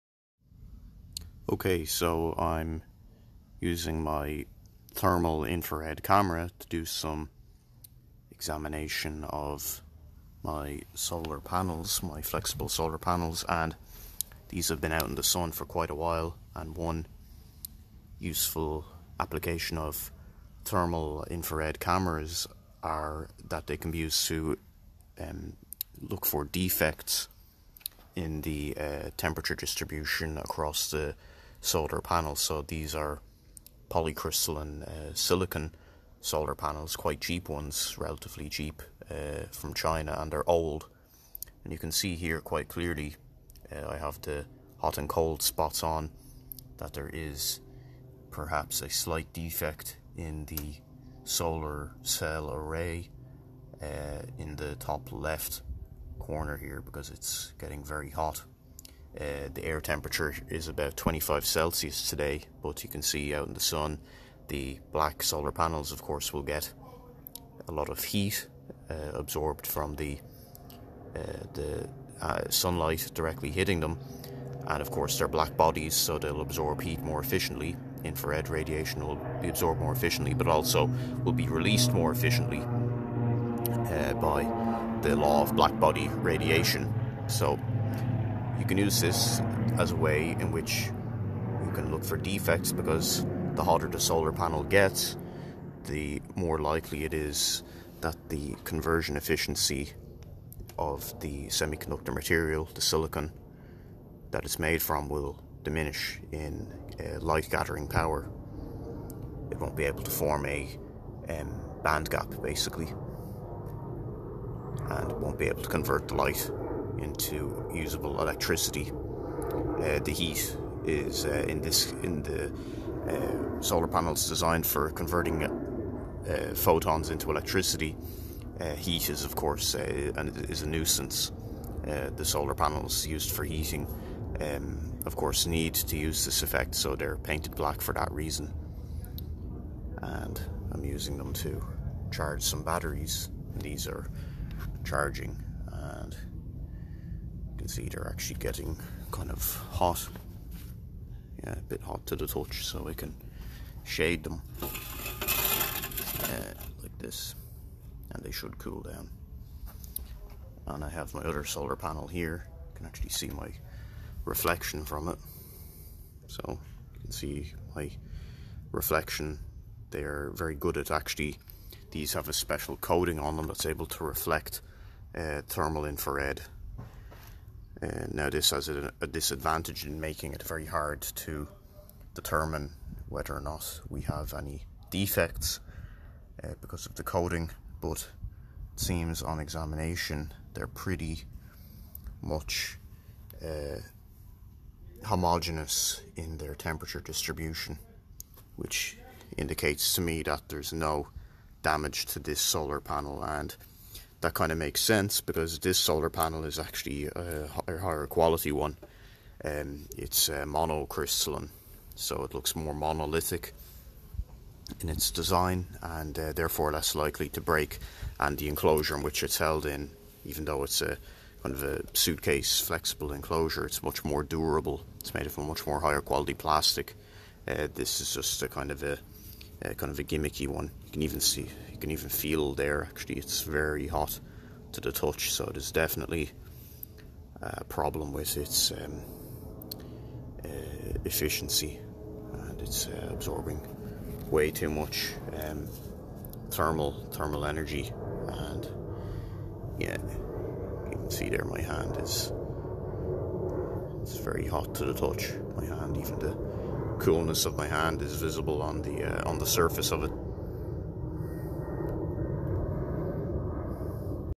Using my FLIR thermal infrared camera to examine my portable solar panels for energy harvesting.
a brief video of me doing some infrared visual inspection of solar cells.